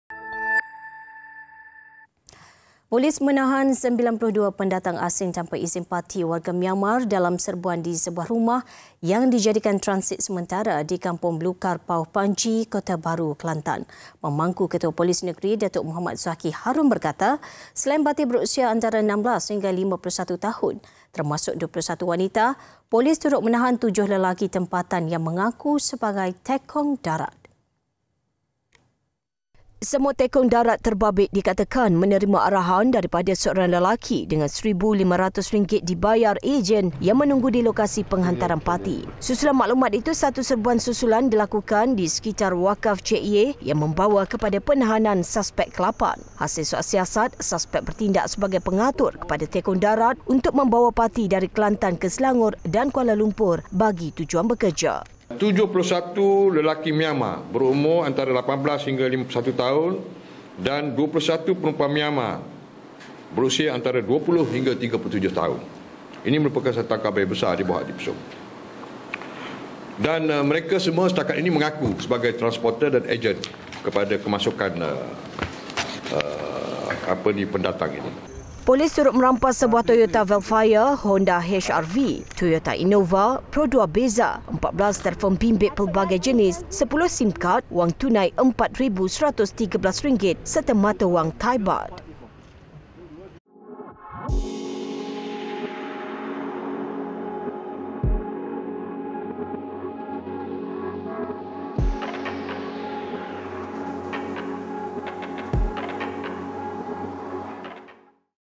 PATI | 92 PATI Myanmar Ditahan, 8 Tekong Darat Dicekup
Polis menahan 92 pendatang asing tanpa izin (PATI) warga Myanmar dalam serbuan di sebuah rumah yang dijadikan transit sementara di Kampung Belukar Pauh, Panji, Kota Bharu, Kelantan.